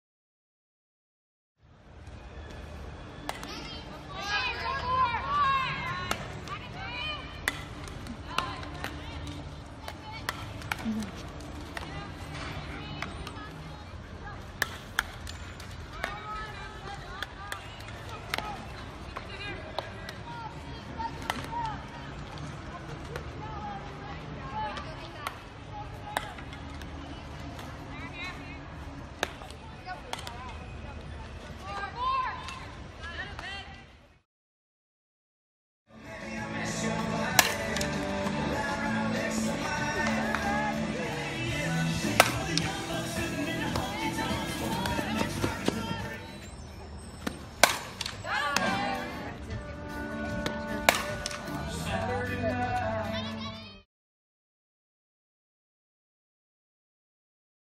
2nd Baseman, Macy Graf, Works Hard At Texas A&M Elite Camp!
Macy Graf, 2nd baseman of Texas Travelers Gold, had a blast at Texas A&M Softball Elite Camp, 9/2018.  Macy looks forward to every college softball camp for the learning experience.  Macy isn’t shy and loves show the college coaches how she works hard!